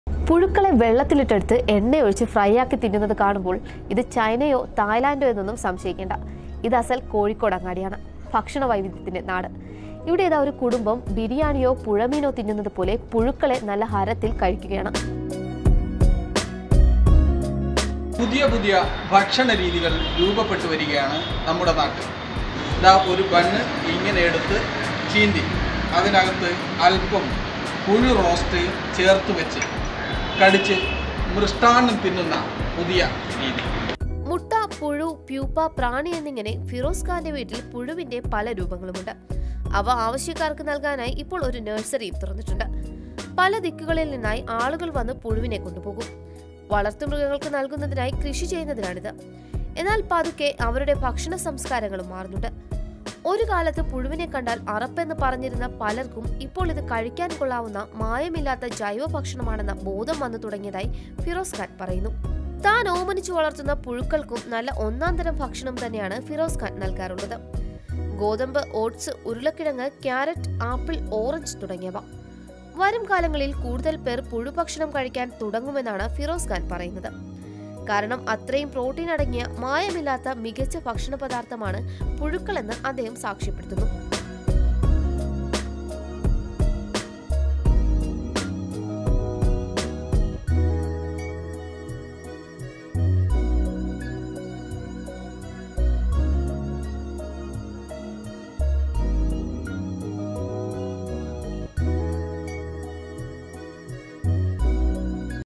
പുഴുവിനെ പൊരിച്ചു തിന്നുന്ന കുട്ടികളെ കാണൂ | Oneindia Malayalam
പുഴുക്കളെ വെള്ളത്തിലിട്ടെടുത്ത് എണ്ണയൊഴിച്ചു ഫ്രൈയാക്കി തിന്നുന്നതു കാണുമ്പോള്‍ ഇതു ചൈനയോ തായ്‌ലന്‍ഡോ എന്നൊന്നും സംശയം വേണ്ട. ഇത് അസല്‍ കോഴിക്കോട്ടങ്ങാടിയാണ്. ഭക്ഷണവൈവിധ്യത്തിന്റെ നാട്. ഇവിടെയിതാ ഒരു കുടുംബം ബിരിയാണിയോ പുഴമീനോ തിന്നുന്നപോലെ പുഴുക്കളെ നല്ല ഹരത്തില്‍ അടിച്ചുകേറ്റുകയാണ്.